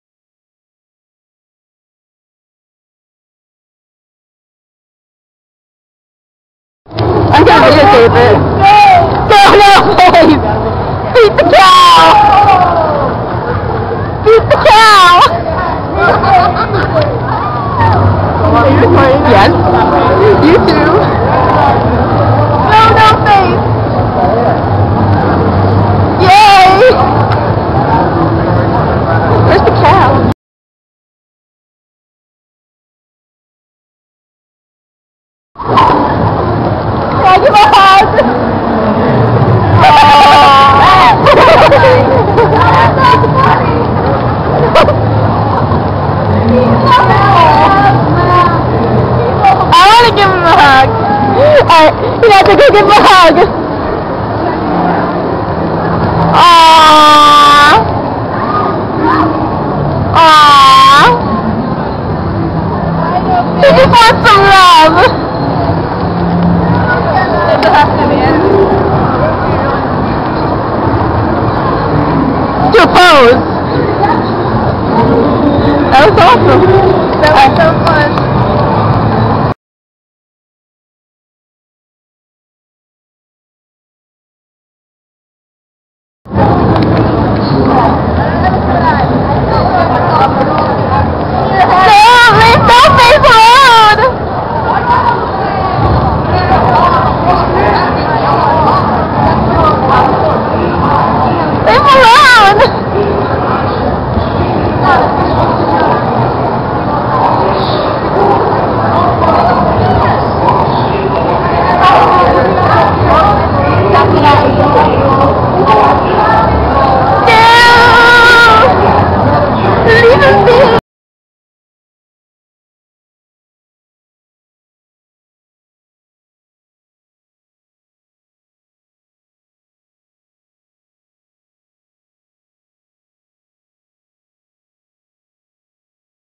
Tribute to No Face
Just in case you don't know, this is No Face from 'Spirited Away'. WE saw him at the New York Anime Festival, and we thought he was really awesome so we just had to video tape him. And, at the festival, there were a lot of yaoi paddles. XD
From ItachiGF and EmXchan